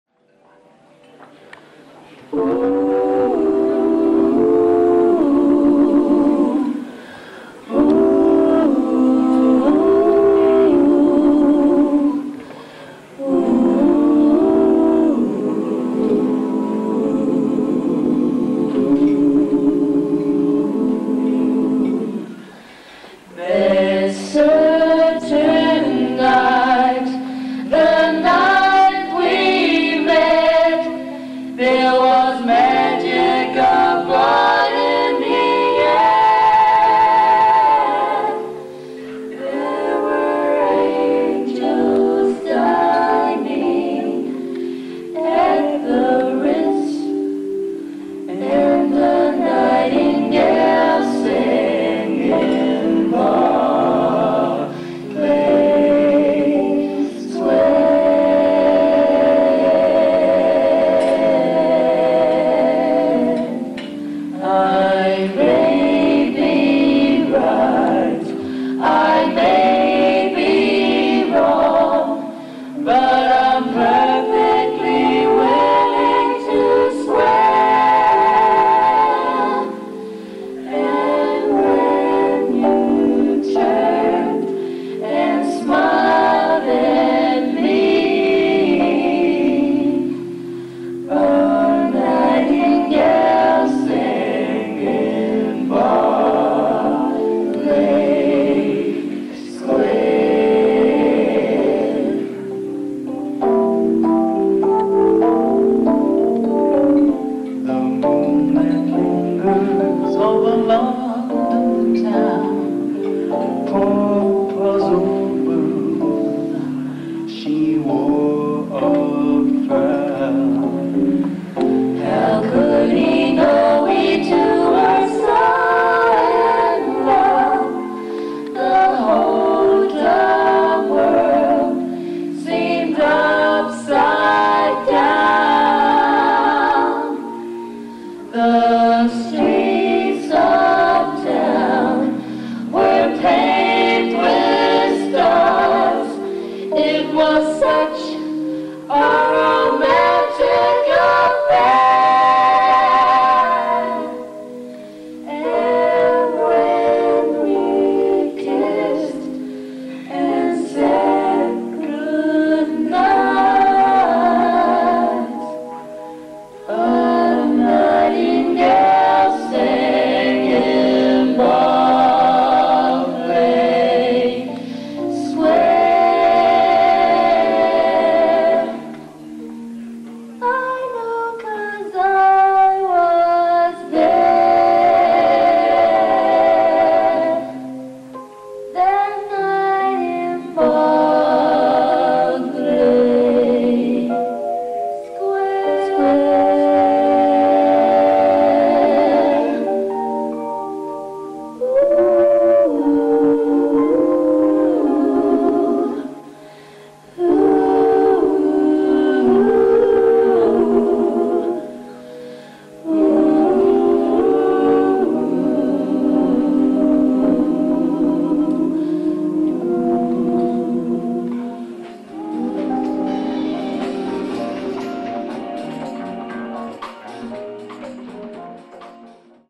Friendship Force CCNZ
Ursula Stockman, Taisha Tari, Leon Wharekura, Quentin Going and Renata Kahuroa on Keys.